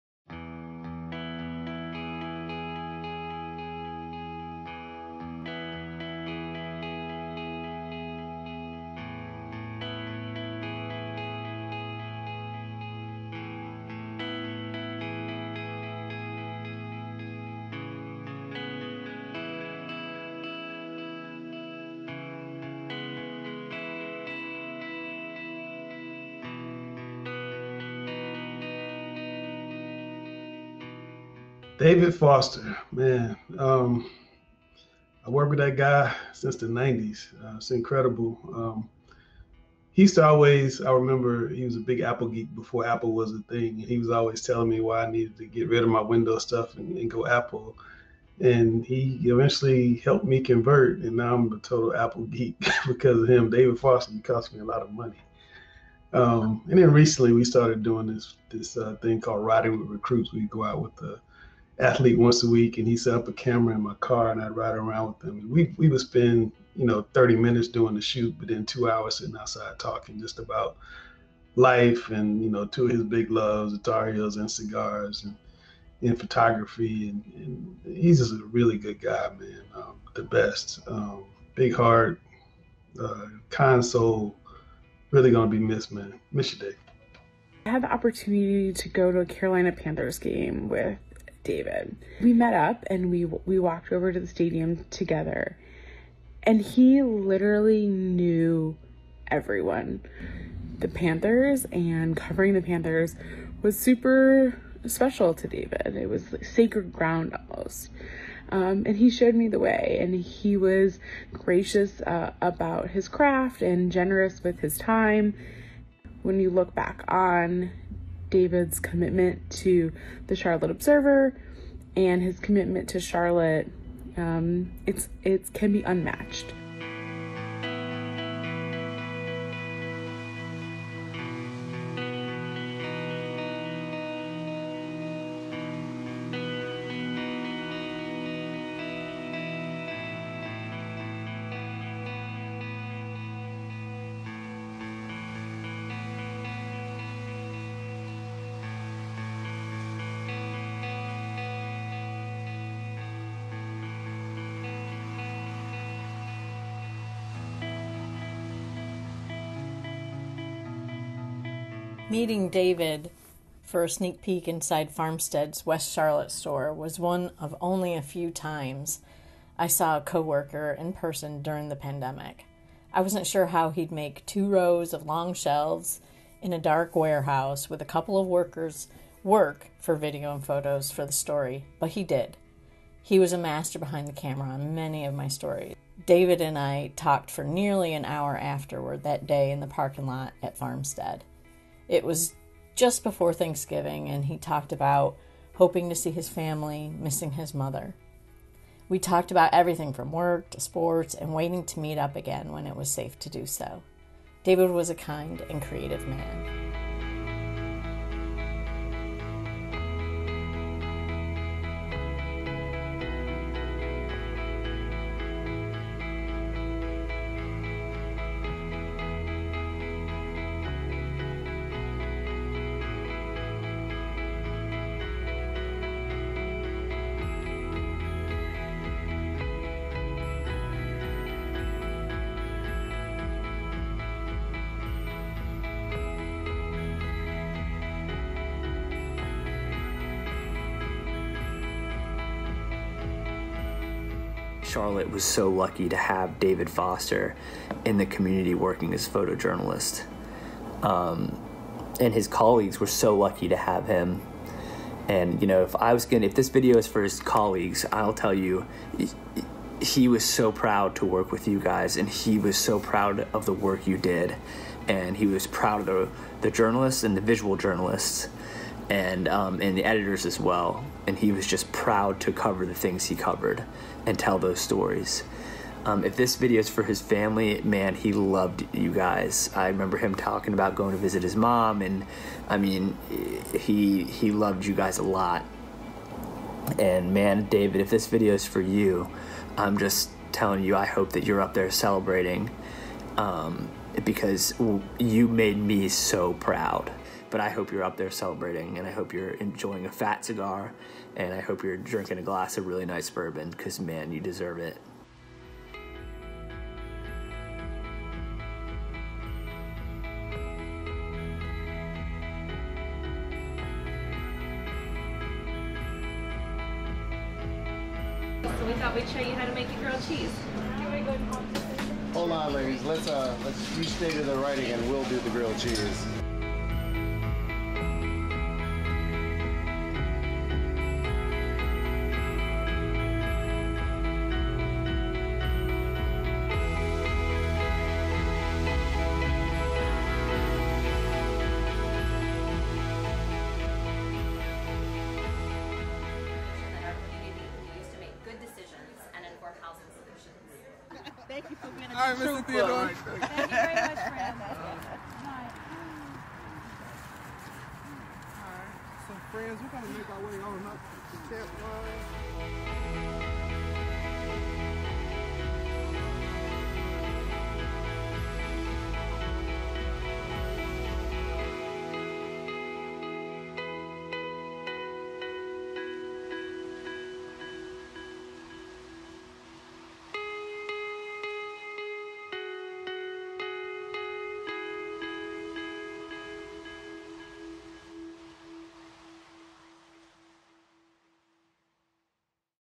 Friends and colleagues remember longtime Observer photographer
Read the obituary: Longtime Observer photographer, ‘one of the great action sports shooters,’ dead at 52

David T. Foster III, the cigar-chomping, UNC Tar Heel-loving bear of a man — a photographer for the Observer and lifelong resident of the Carolinas — was found dead in his apartment on Monday, May 24, 2021.
Video by Kevin Keister / The Charlotte Observer